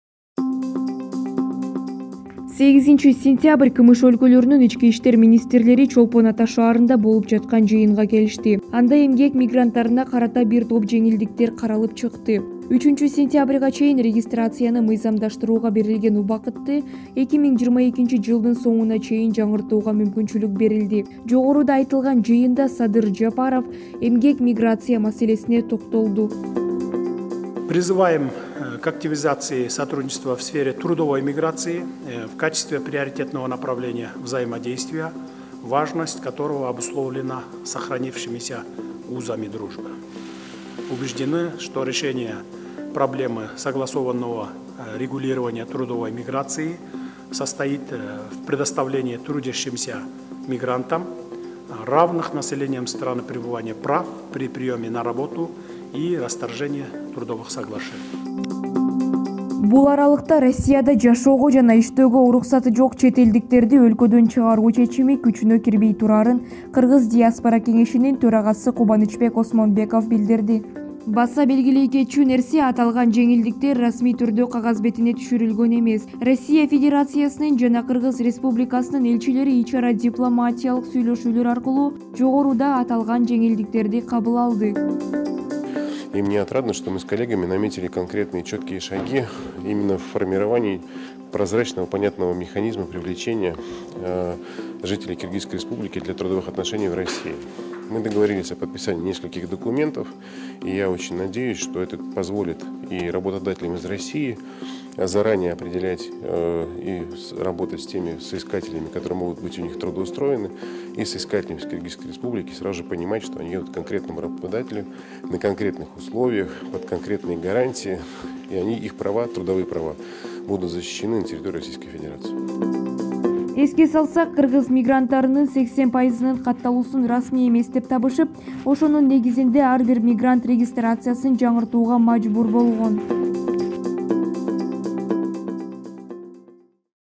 2022-жылдын соңуна чейин мигранттар эч кандай тоскоолдуксуз жашоого мүмкүнчүлүк алышты.
Эмгек мигранттарына расмий эмес каттоосу менен 3 ай Орусияда эч кандай кыйынчылыгы жок жашоого уруксат берилди. Бирок, бул келишим 2022-жылдын соңуна чейин гана күчүндө болот.